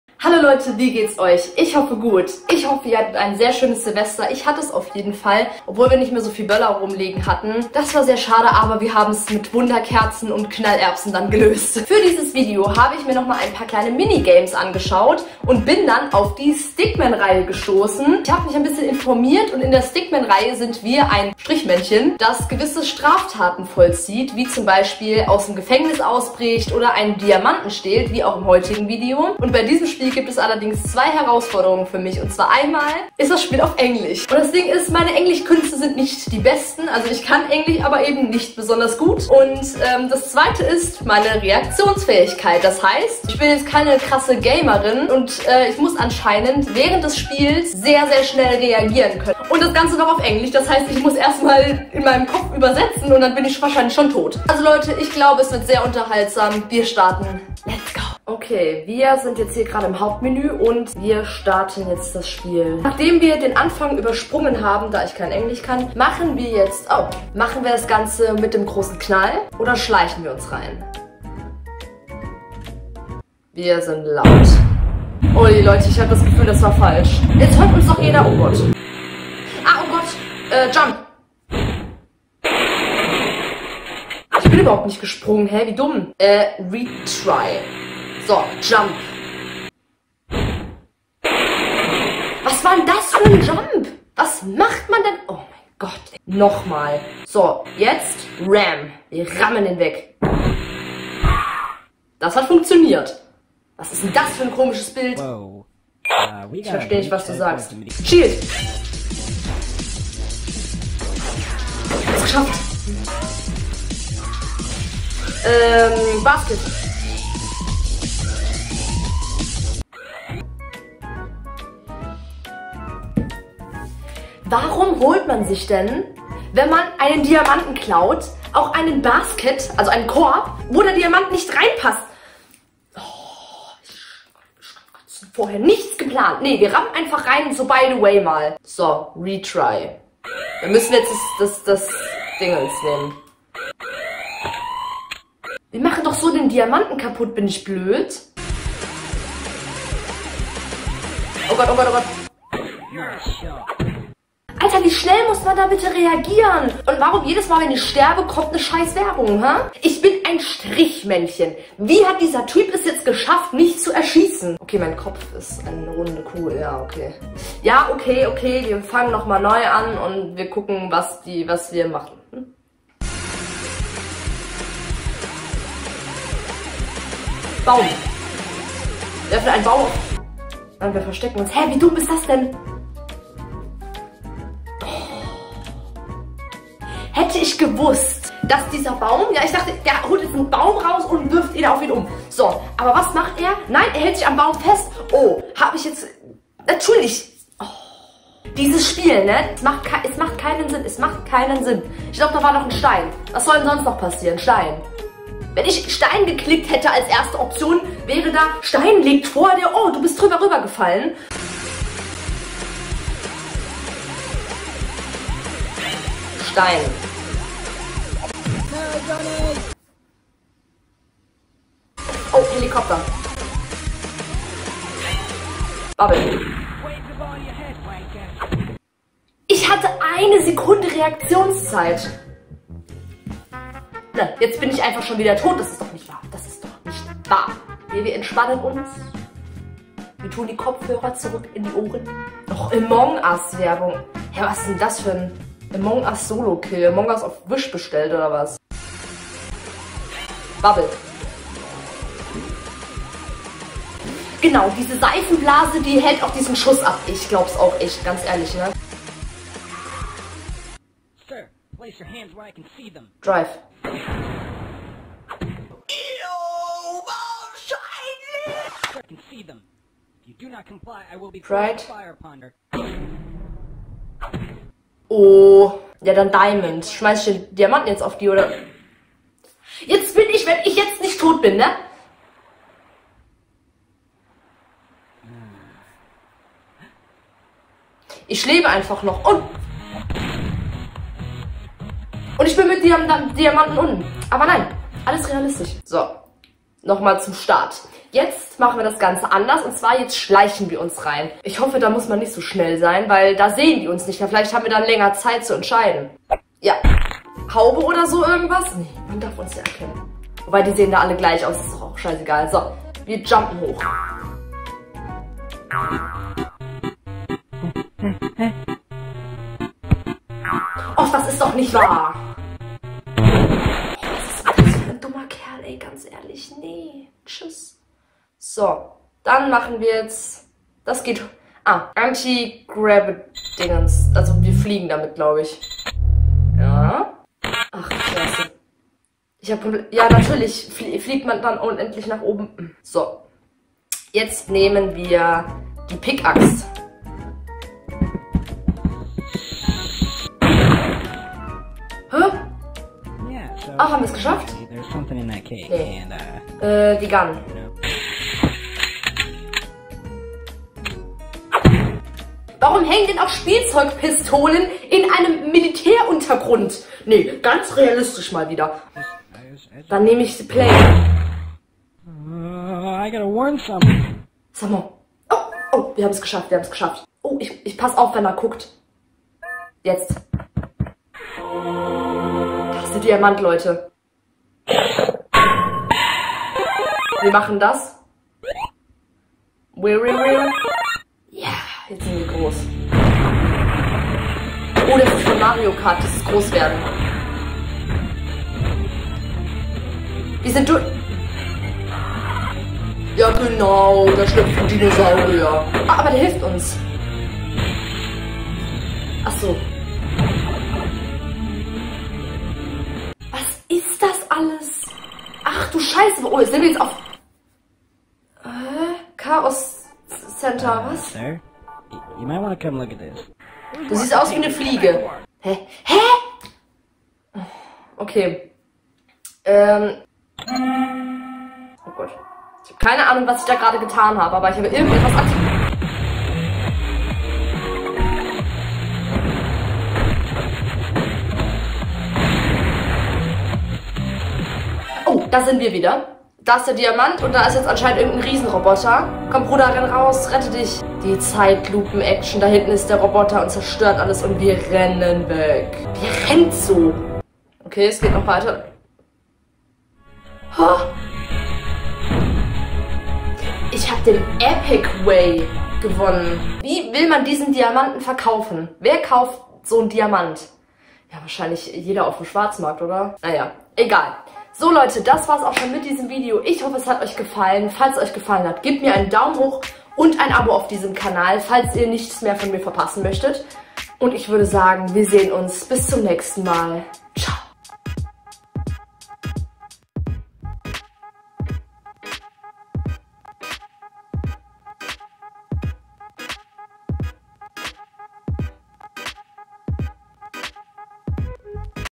Ich stehle einen Diamanten!
Hallo Leute, heute spielen wir ein neues Minigame!
E ist eins aus der Stickman-Reihe.
Viel Spaß!